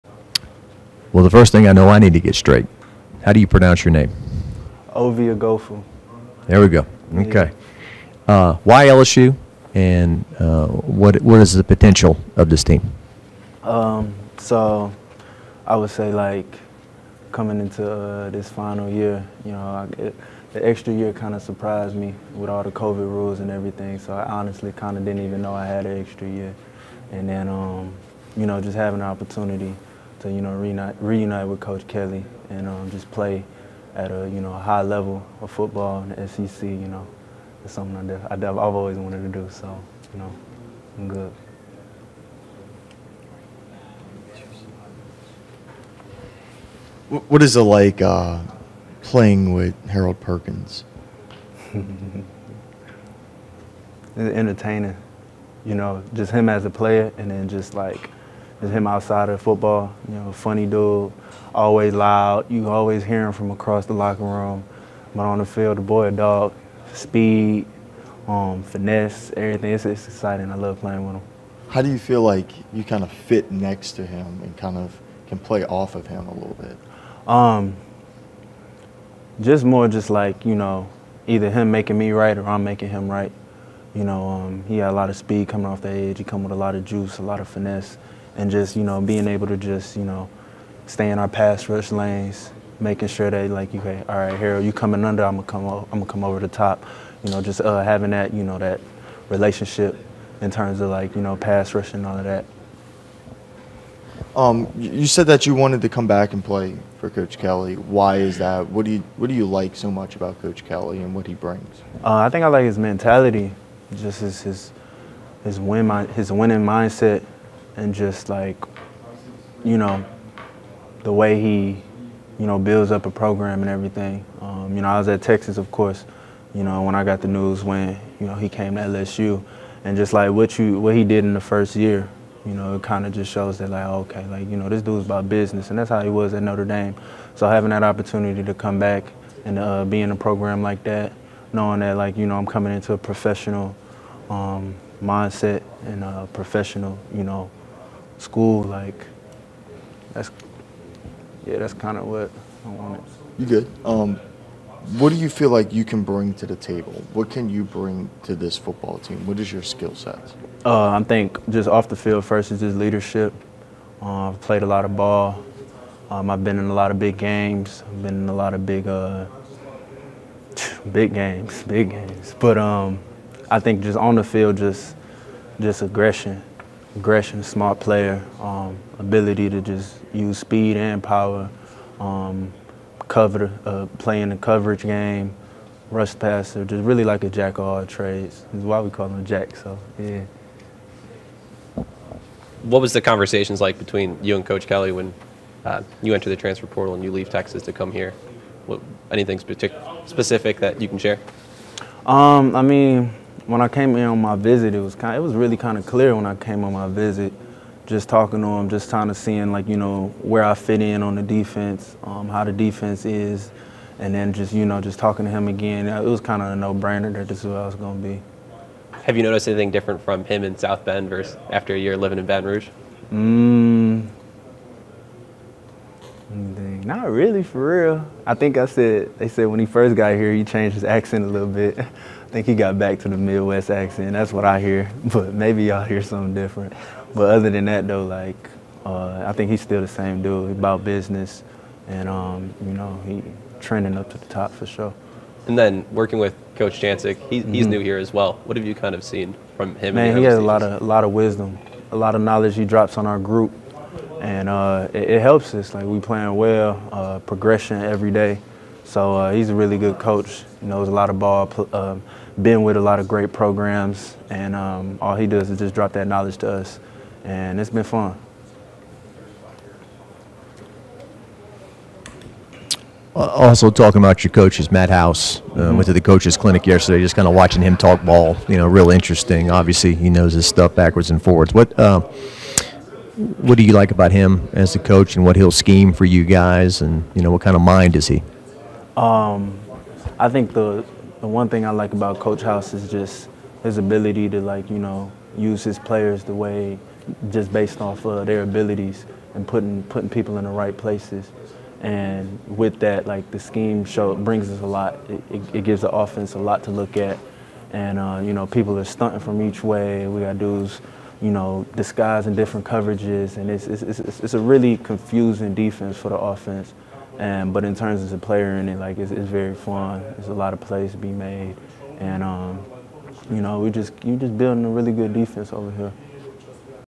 Ovie Oghoufo on playing with Harold Perkins, leading LSU Football, reuniting with Brian Kelly
Defensive end Ovie Oghoufo on playing with Harold Perkins, leading LSU Football, reuniting with Brian Kelly.

LSU Tigers Football videos from Baton Rouge, Louisiana from the staff at The Bengal Tiger On3